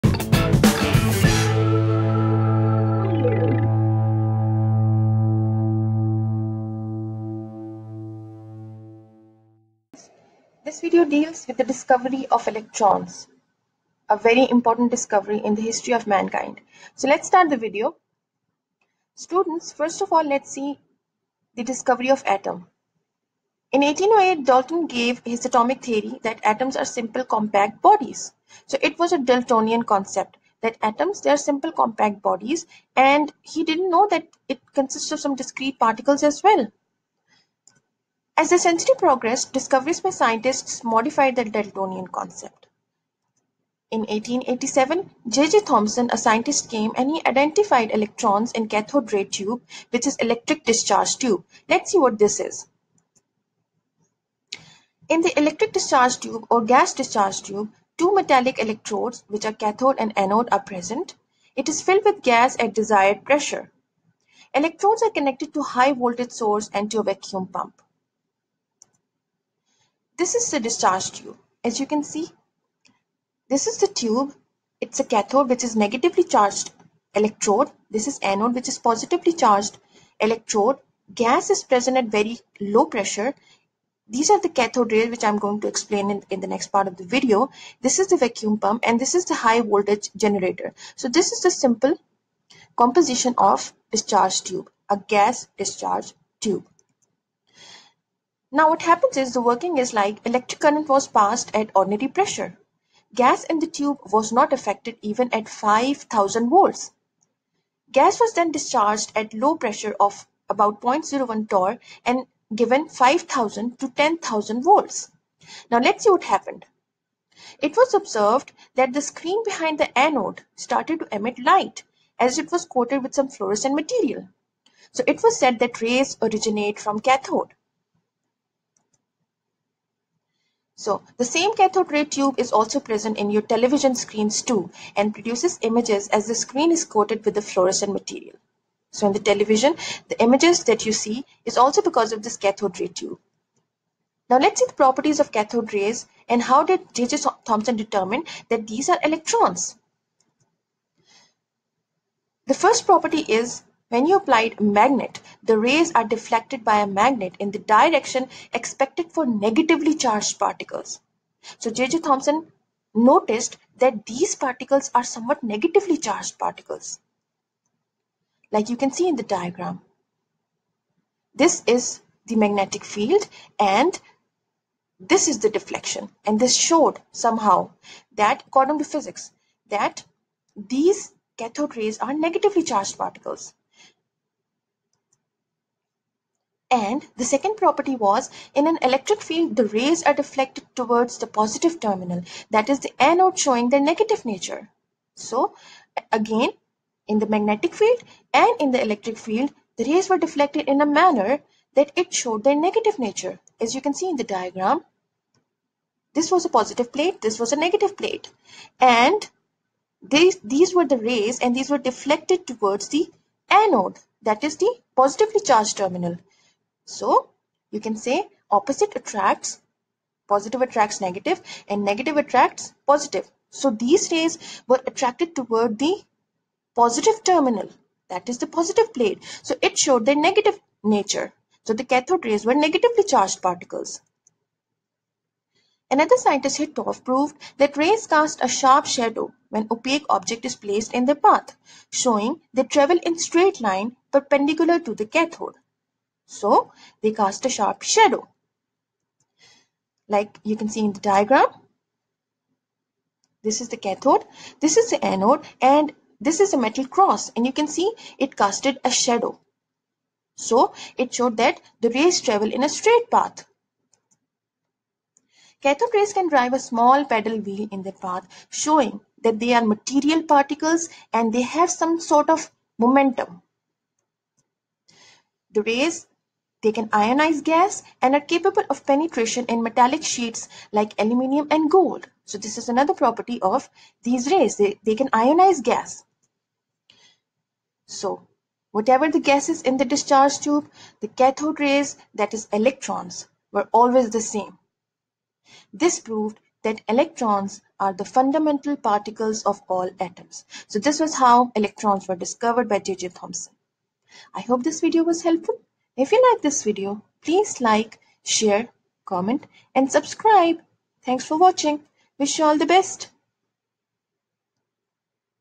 Discovery of electrons : ( Easy learning ) :
This video tells that how electrons were discovered . Many scientists played their role but J.J Thompson was the person who identified that electrons are the fundamental particle of atoms . He used gas discharge tube which had cathode and anode as negative and positive electrodes respectively . It has gas at very low pressure and was connected to a high voltage source . The gas was discharged at very low pressure and very high voltage was applied and some glow was observed behind the anode o the screen covered with some fluorescent material. He observed some properties of the rays travelling from cathode to anode . In the magnetic and electric field the rays were deflected in a manner showing the negative nature of these rays . Despite of the gas used in the tube the cathode rays were always the same . So J.J Thompson concluded that these rays consist of negative particles which he termed as electrons and these are the fundamental particles of the atoms .

follow me